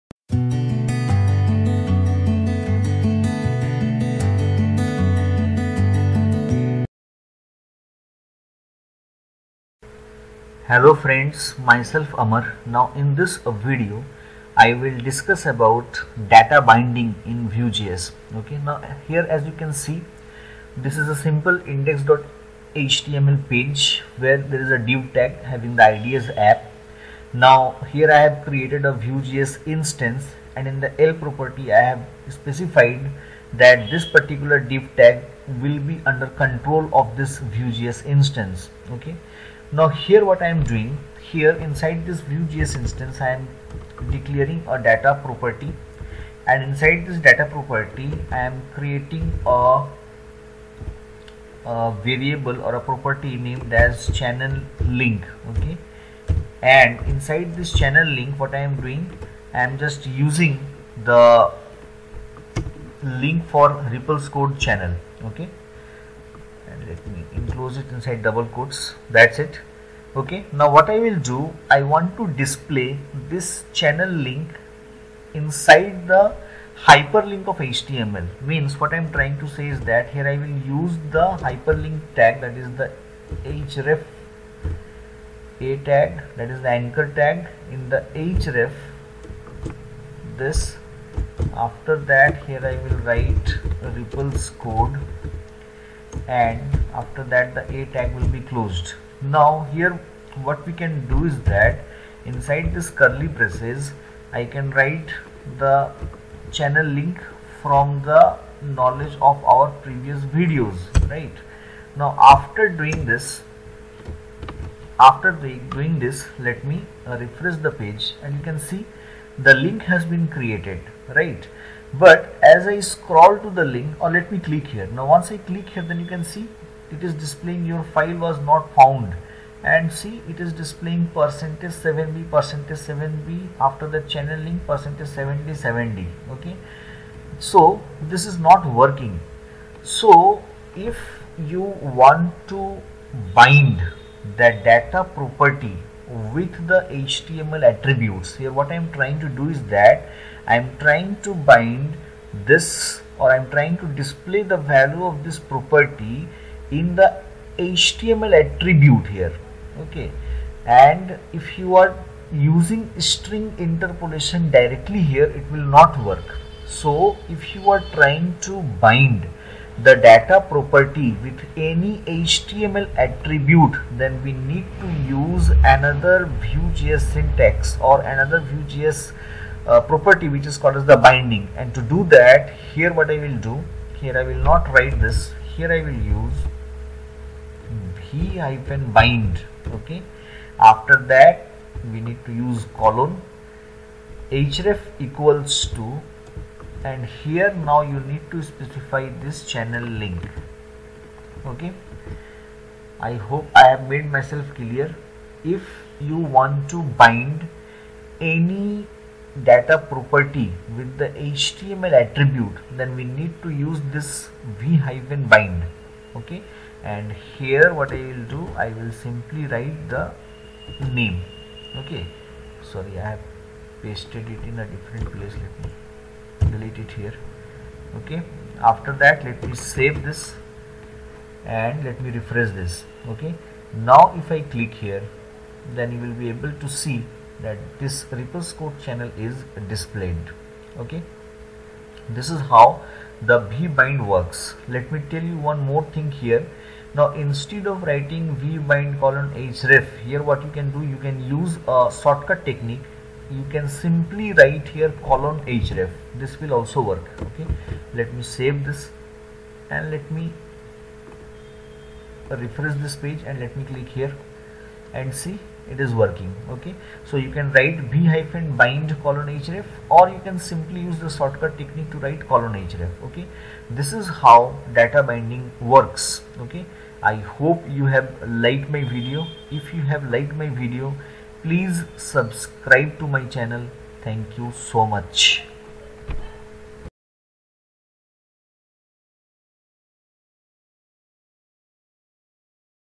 VueJS 2 Series including Router & Vuex || How to do Data Binding using v-bind directive || Part-4
In this video  I have discussed about Data Binding using v-bind directive.
If we want to bind values with any html attribute then v-bind directive is used.
Directive are instruction given to VueJS framework  to perform certain task in a specific way.